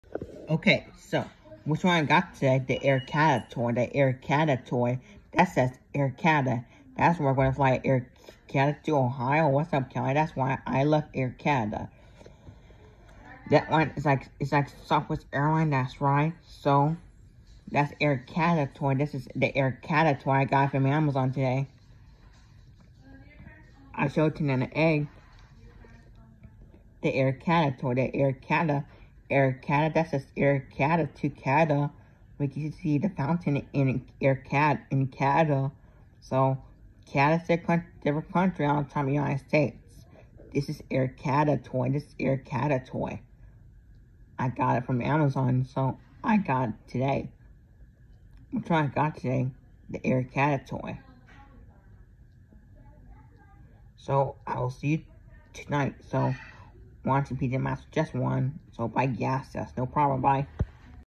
I have Air Canada 🇨🇦 toy 🧸
I got it today from Amazon from Vivian so thank you 😊 very much we can fly Air Canada 🇨🇦 Ohio that’s why Air Canada 🇨🇦 to Canada 🇨🇦 we have it in Houston Texas at George Bush international airport of Houston Texas. We have Canada 🇨🇦 well fly Air Canada 🇨🇦 to Ohio I love  ❤️ Air Canada  🇨🇦 Canada  🇨🇦 different country on top  🔝 United States  🇺🇸 ￼